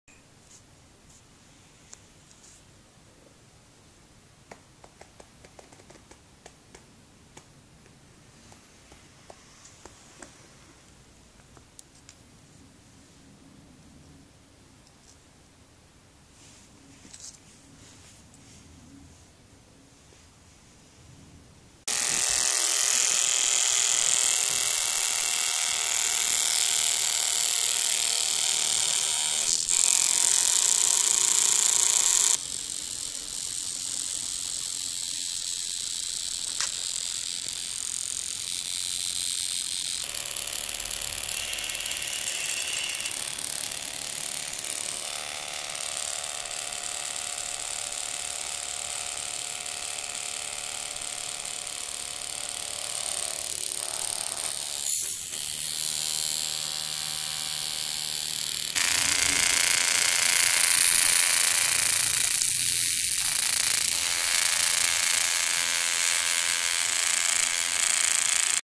ScaraBot Catamarano
robot creatore di scarabocchi a forma di catamarano realizzato con materiali recuperati; questo fatto due con cucchiai / scribbles machine robot catamaran style, made with recovery materials; this bot is made by two spoons